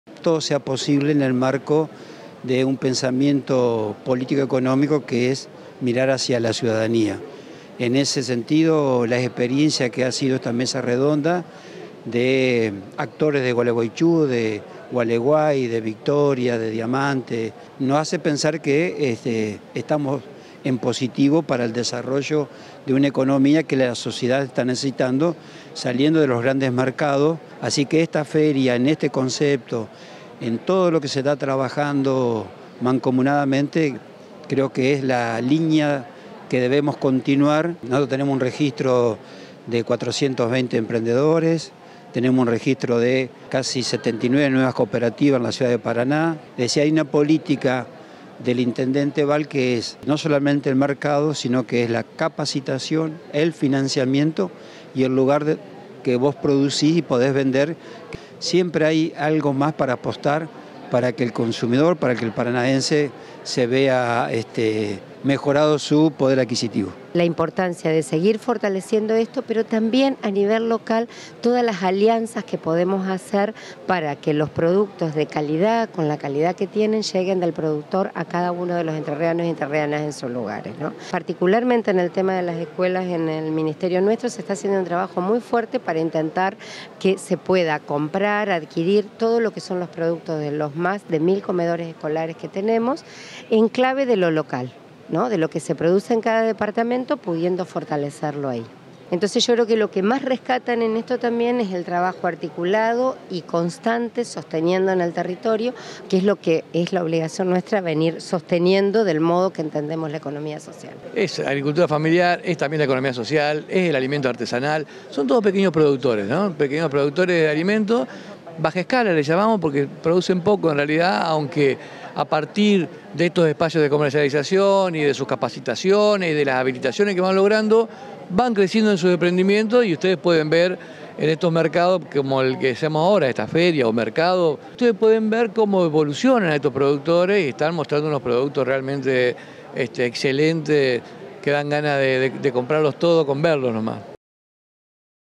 Se realizó el Mercado Provincial de Alimentos
Fue en la Sala Mayo este viernes y sábado, con más de 80 emprendimientos de distintos puntos de la provincia. El espacio fue impulsado en conjunto por el Gobierno provincial, el Instituto de Control de Alimentación y Bromatología (ICAB) y la Municipalidad de Paraná